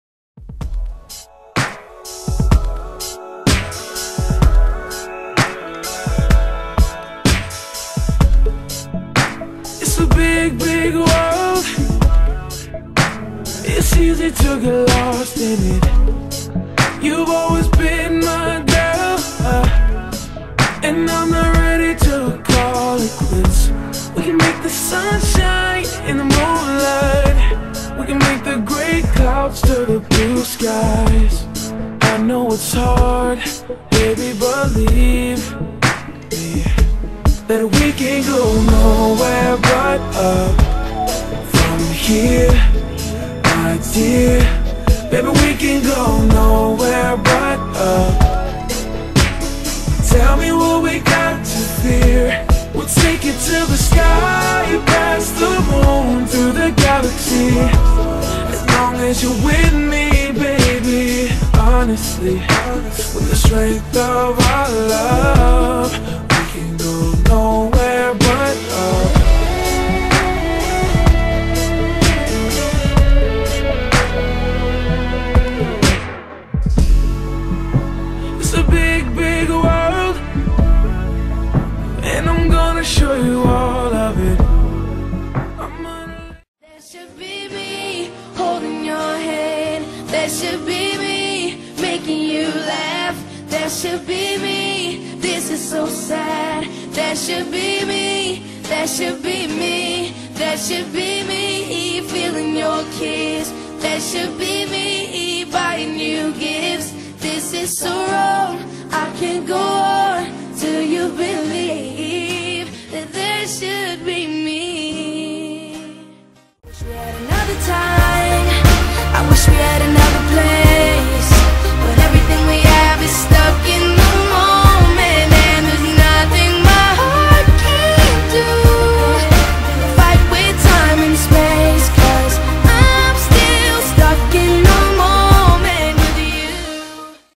10 reason to love justin bieber
10 Reasons to love the cutest canadian Justin Bieber,